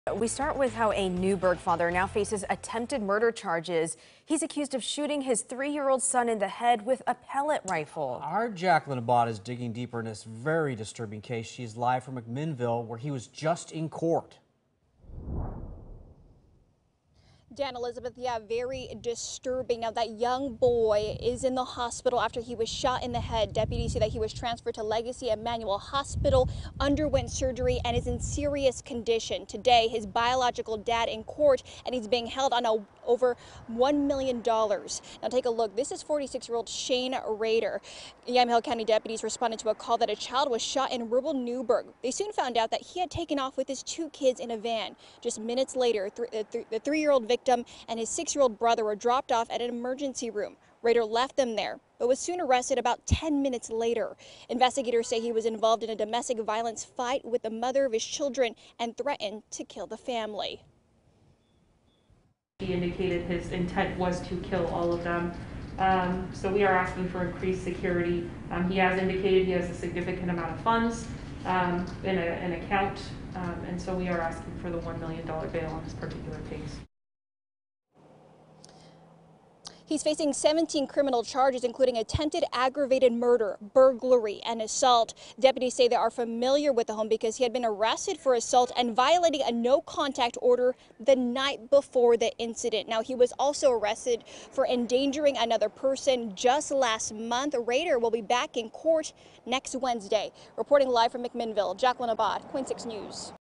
Man arrested for attempted murder of 3-year-old son
A Newberg man has been charged with attempted murder after allegedly shooting his 3-year-old son in the head.